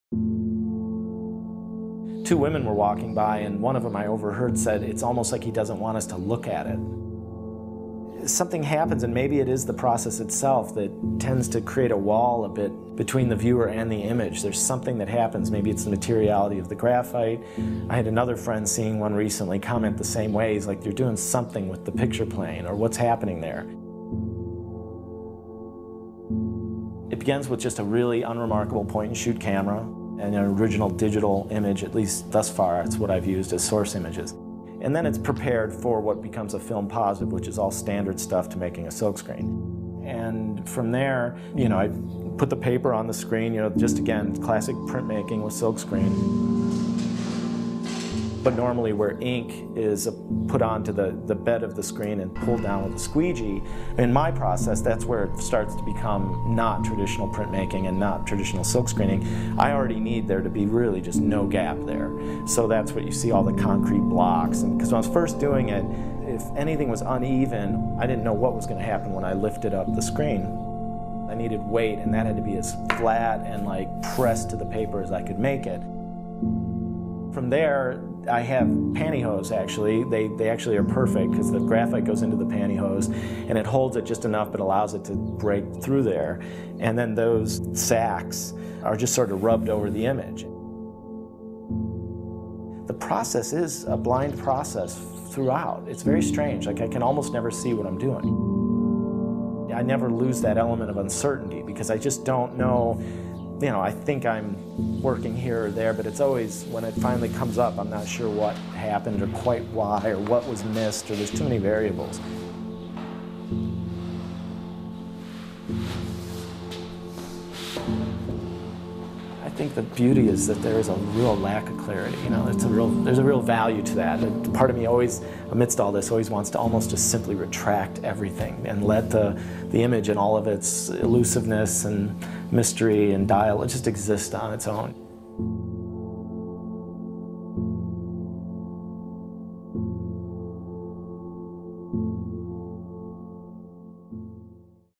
Artist T.R. Ericsson's Studio
T.R. Ericsson is one of fifteen artists featured in the exhibition Graphite at the Indianapolis Museum of Art.  In this video, he describes his studio process for creating works in powdered graphite. Two works of art from the Étant Donnés 2° series are included in the exhibition.

 Graphite presents a glimpse into recent and innovative uses of graphite. A form of crystallized carbon, graphite is a naturally occurring mineral as well as a synthetic, industrial product that can be processed and in multiple ways. It can be machined or carved, used as a powder or liquid, or handled as a lump, stick, or pencil. Graphite gathers together works that reveal the material's potential to take a variety of forms and yield a wide range of visual effects.

The first major museum exhibition to explore graphite as a medium in works beyond drawings, Graphite includes sculpture, drawing, and installations created over the past decade—including several newly commissioned works—by emerging and established artists.